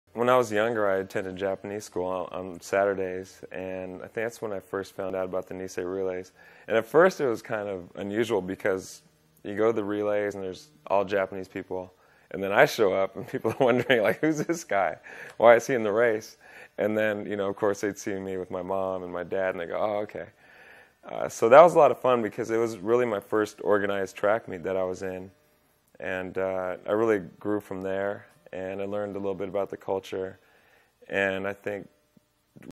Johnnie Morton interview #1: Gained appreciation of his multiracial heritage
Date: February 7, 2000
Location: California, US
Interviewer: John Esaki

Originally published on DiscoverNikkei.org